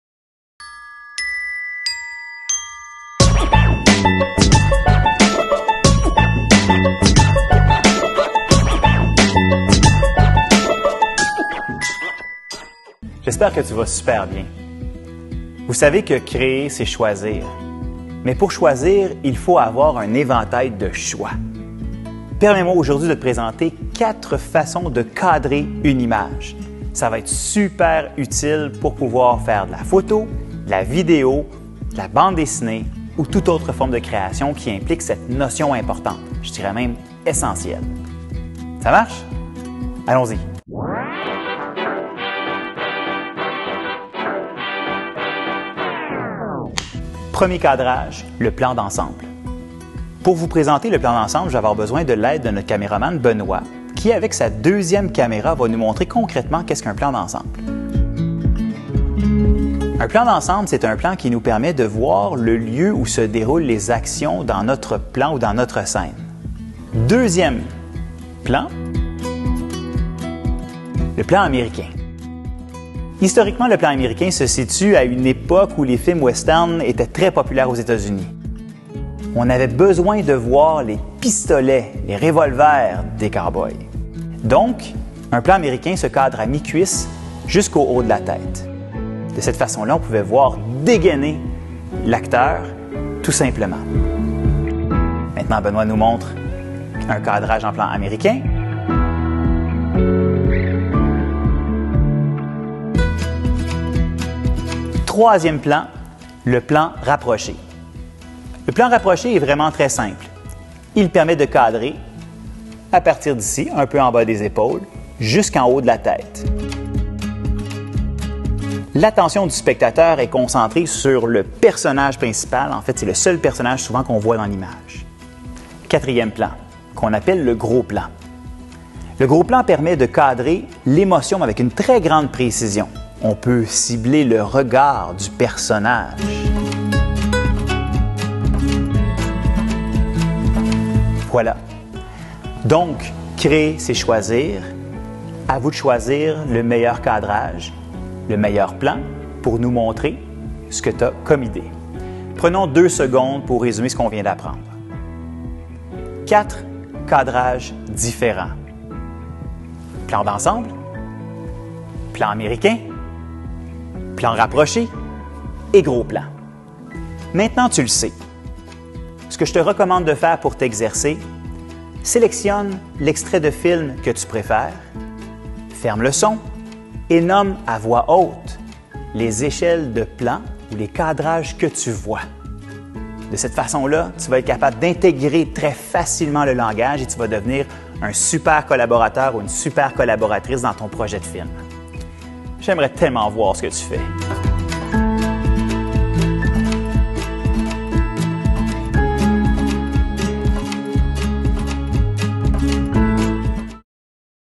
CADRAGES
Voici une capsule explicative concernent le langage multimédia :  différents cadrages lors d'un tournage ou d'un montage vidéo.
Capsule didactique de type classe inversée a été réalisée pour le cours de formation à distance (FAD) de 4e secondaire en arts plastiques